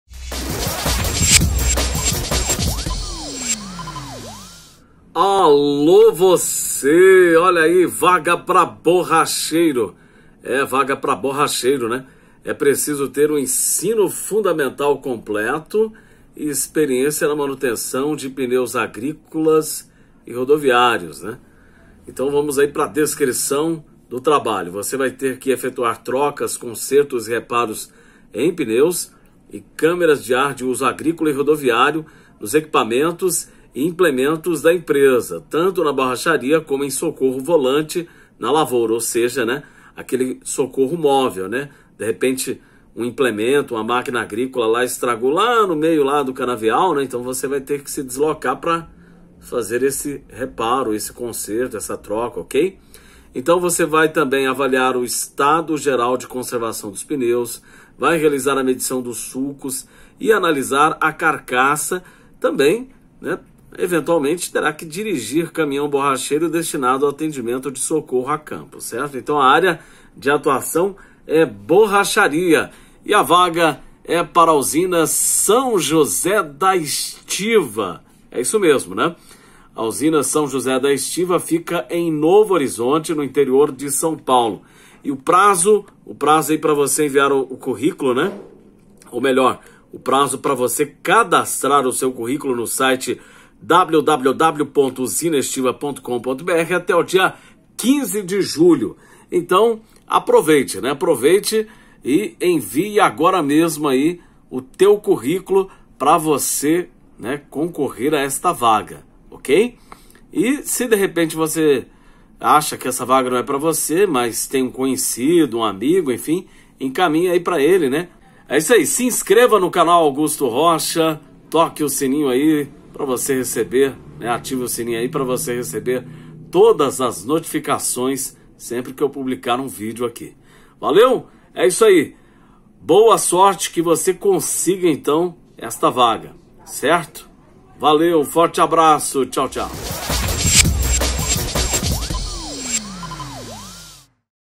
Vaga de emprego para borracheiro em usina de cana-de-açúcar no município de Novo Horizonte - SP
Olá!
Neste vídeo, você vai saber diversas informações sobre uma vaga de emprego para borracheiro na Usina São José da Estiva, no município de Novo Horizonte, no estado de São Paulo!